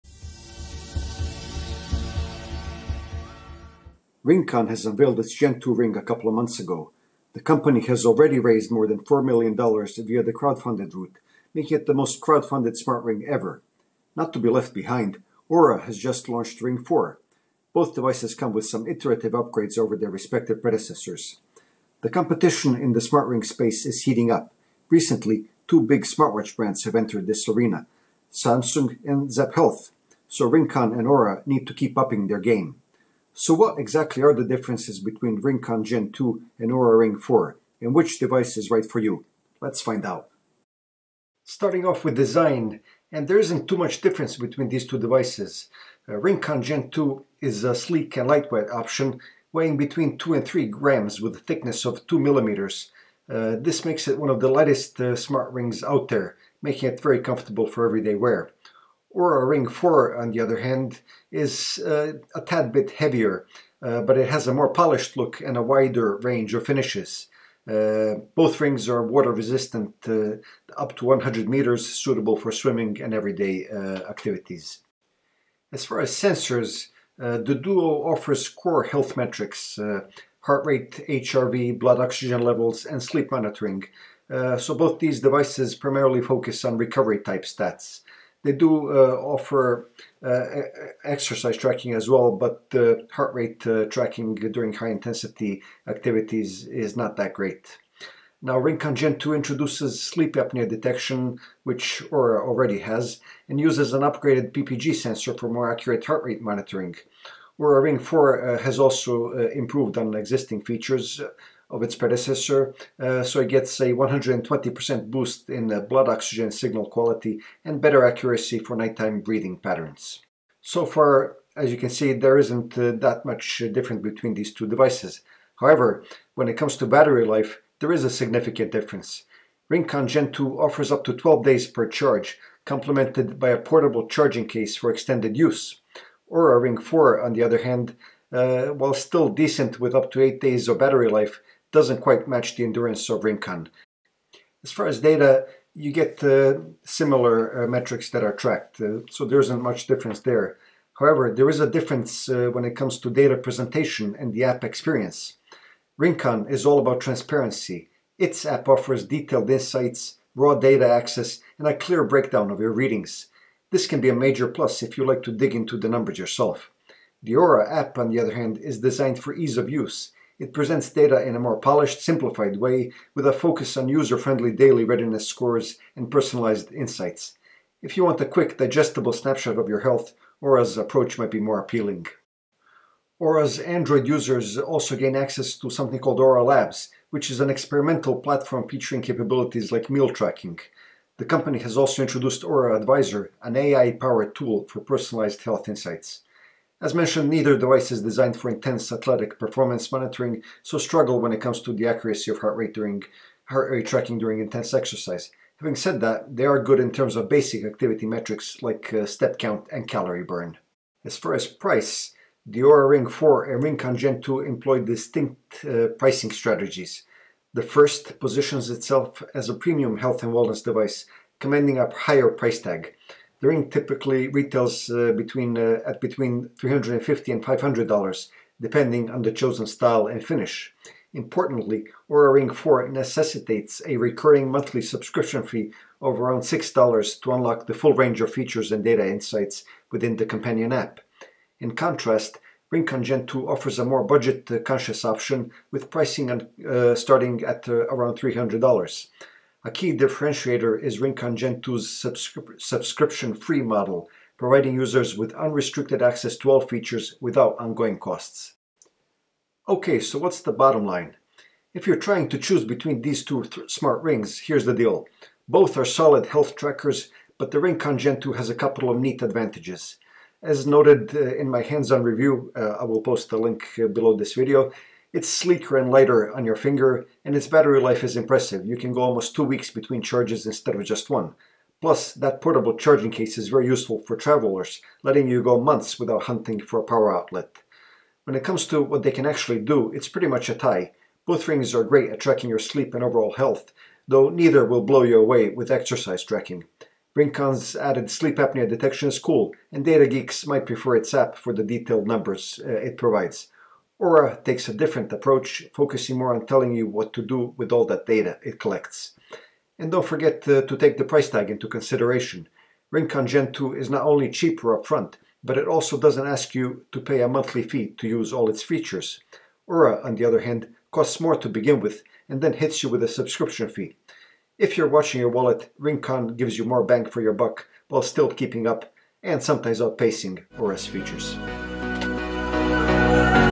Oura Ring 4 vs RingConn Gen 2: Which to go for?
RingConn has unveiled its Gen 2 ring a couple of months ago. The company has already raised more than $4 million via the crowdfunded route, making it the most crowdfunded smart ring ever. I recently posted my full hands-on review of the gadget. Not to be left behind, Oura has just launched Ring 4. Both devices come with some iterative upgrades over their respective predecessors.

0:00 Introduction
0:39 Design differences
1:14 Sensor technology
2:00 Battery life
2:24 Data, smartphone app
3:30 Price
4:18 Bottom line